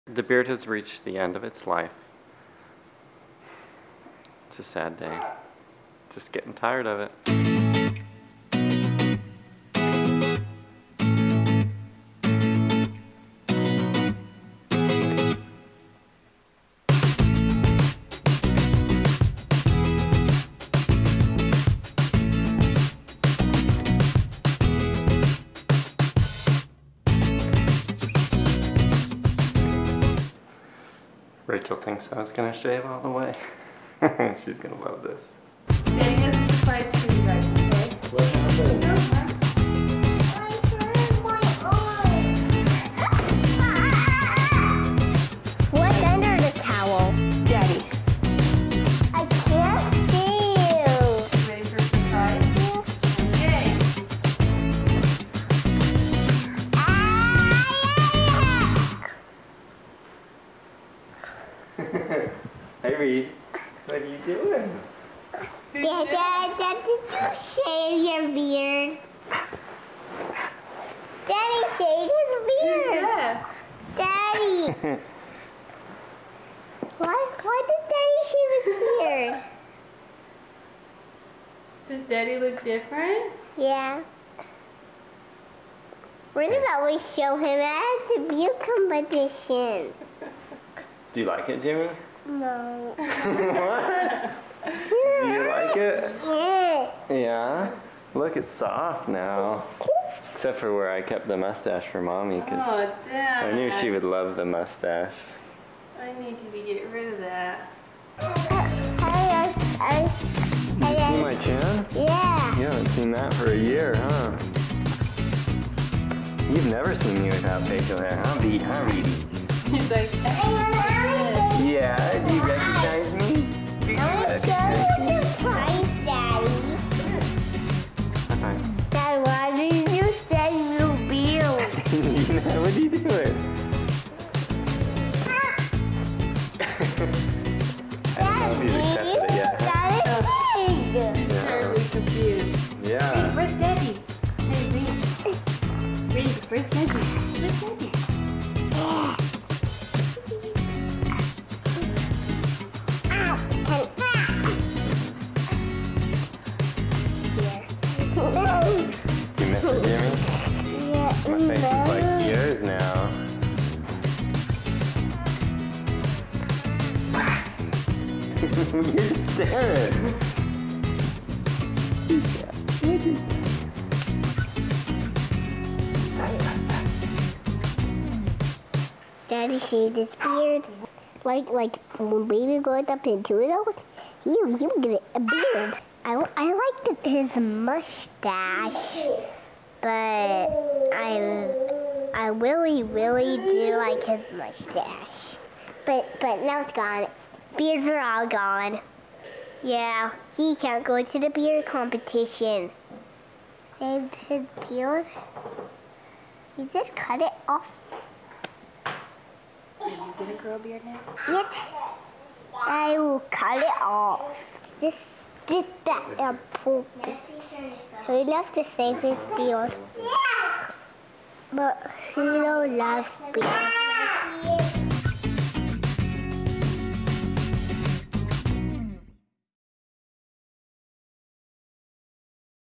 Jim Shaved! The kids reactions were priceless.
Jim decided to shave his "yeard" (beard grown for a year). All of the kids reaction was adorable. Reed has never seen Jim without facial hair and seeing him was priceless.